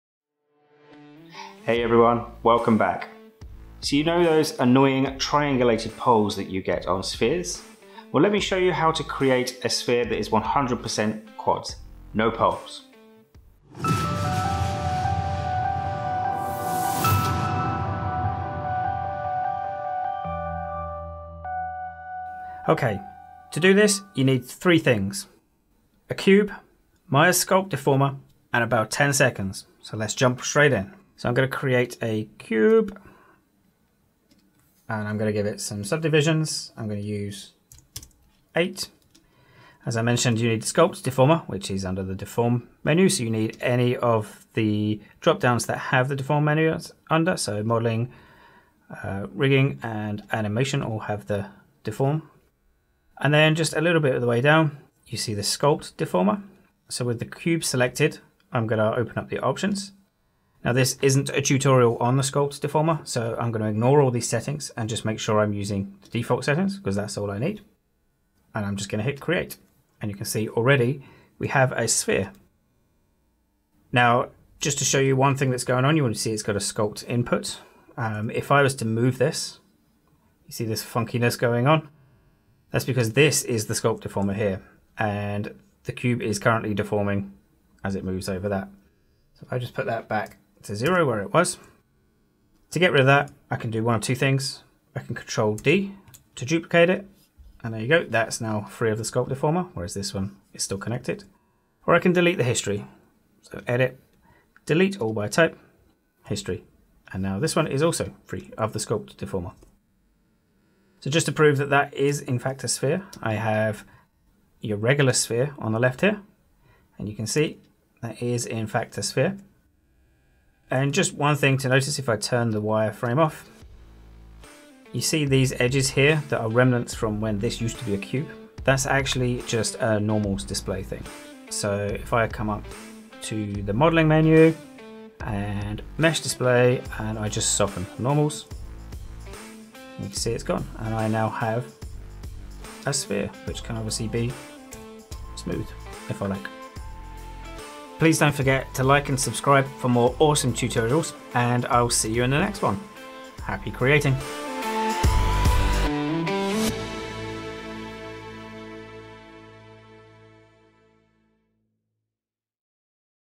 Maya - Quad Sphere
Triangulated poles on Spheres are so annoying, In this super quick tutorial I'll show you how to create a sphere that is 100% quads in seconds.